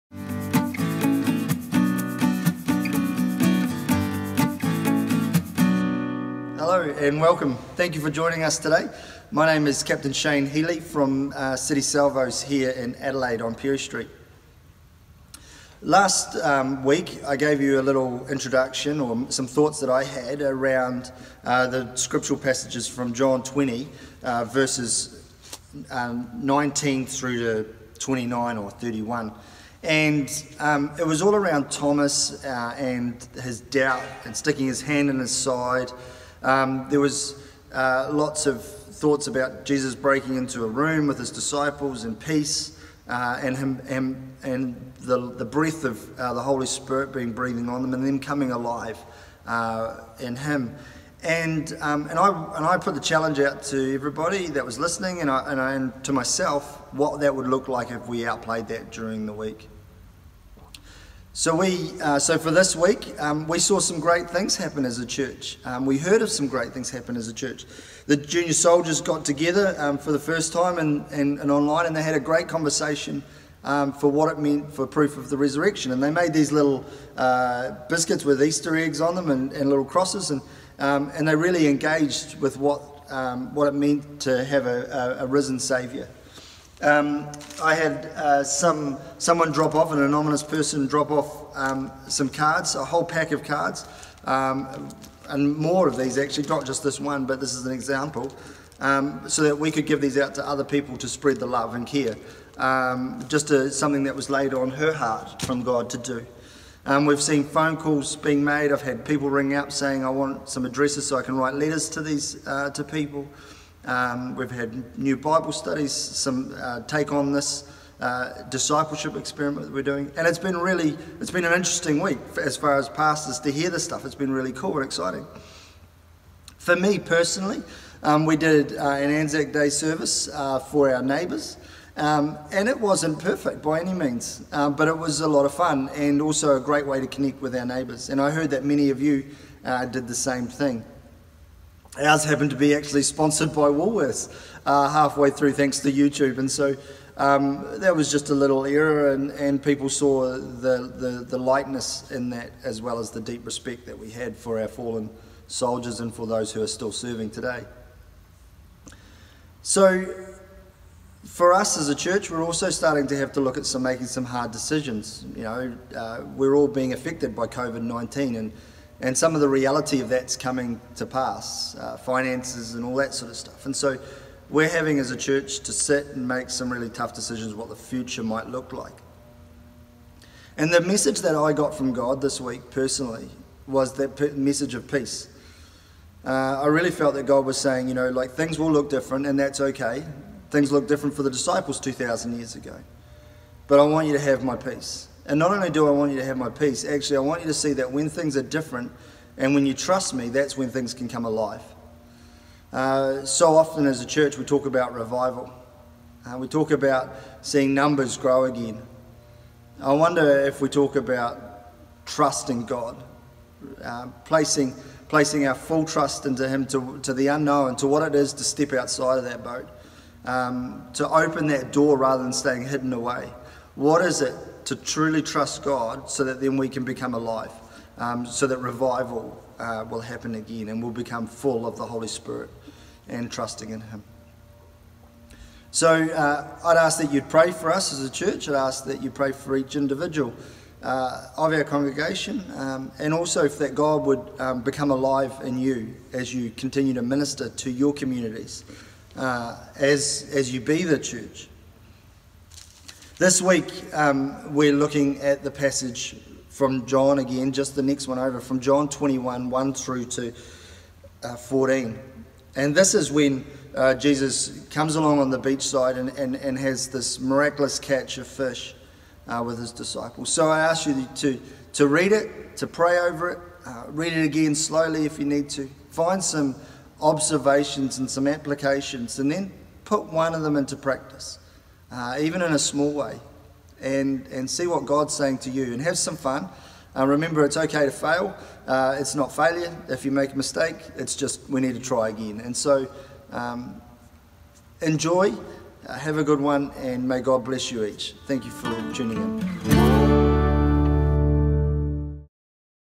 ACH - Discipleship Experiment - John 20:19-29 Part Two
In this video Captain Shane finishes his thoughts on this passage - how he applied what he had learned and what impact this has had on his life.

He also introduces the passage for the coming week  - John 21:1-14